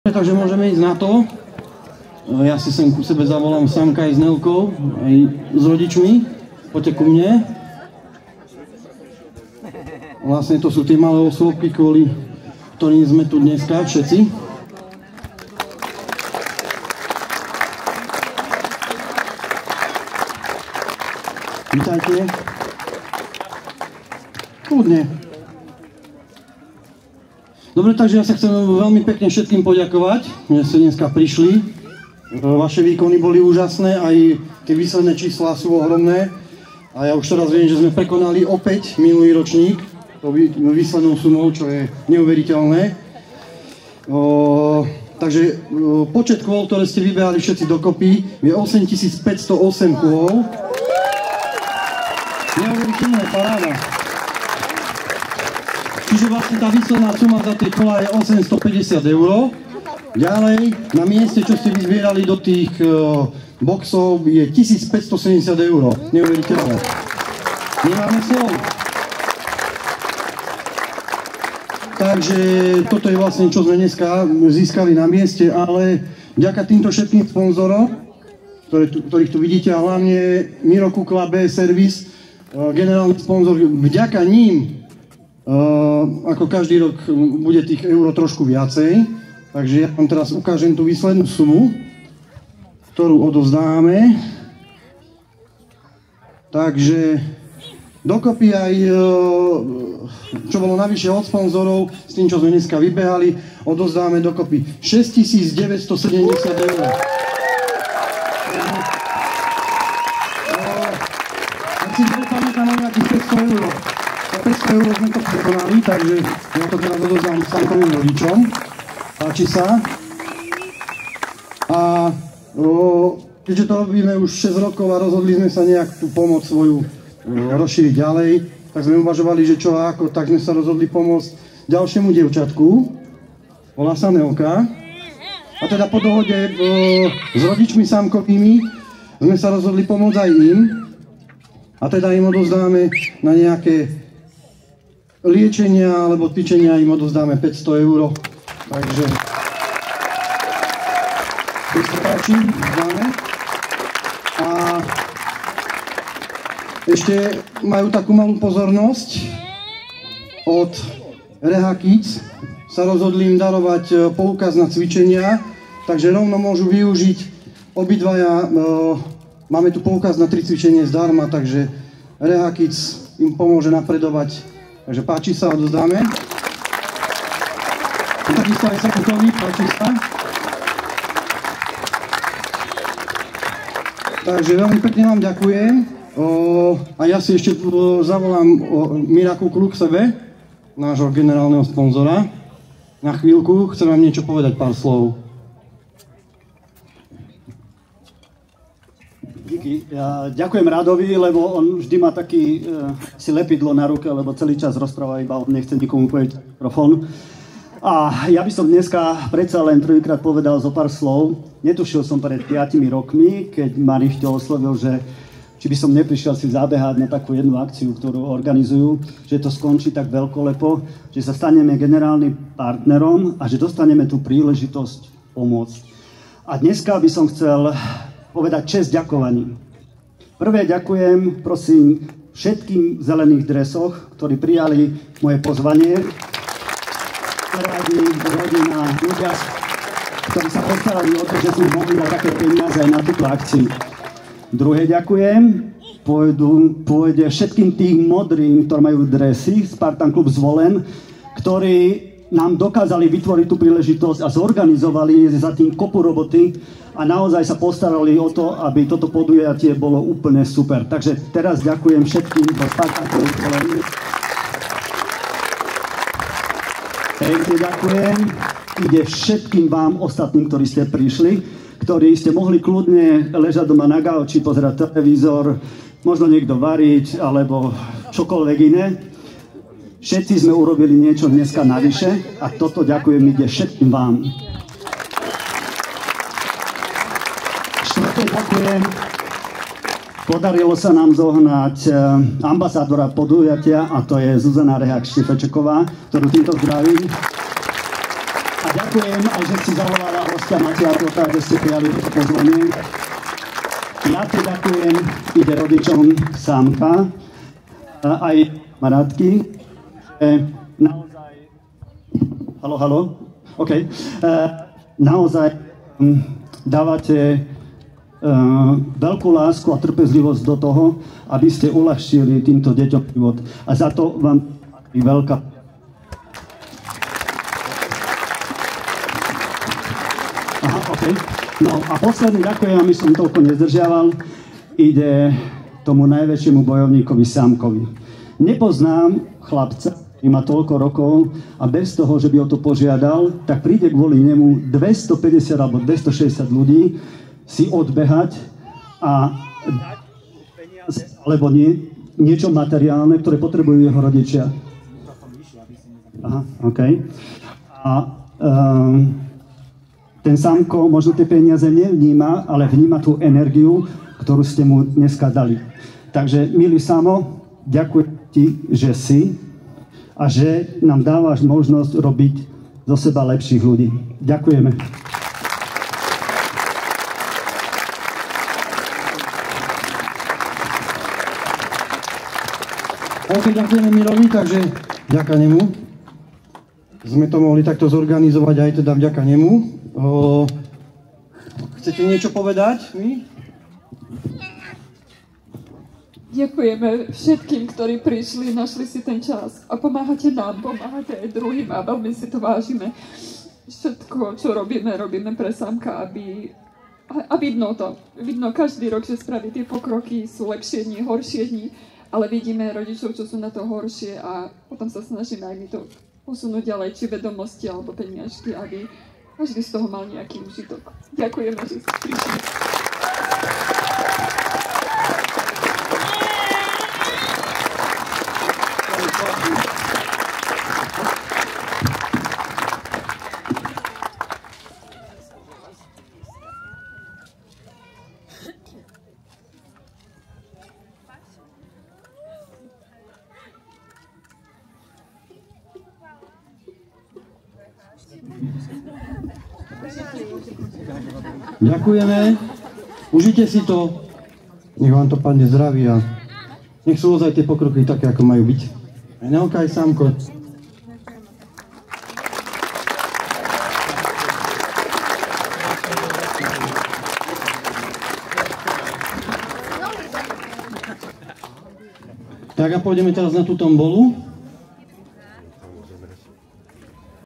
Hodinovka pre Samka 2024 (vyhodnotenie)
V nedeľu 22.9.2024 sa v športovom areáli základnej školy Hrnčiarskej vo Zvolene konal už v poradí 6. ročník charitatívneho podujatia Hodinovka pre Samka.

Na záver sa konalo slávnostné vyhlásenie výsledkov a odovzdávanie cien.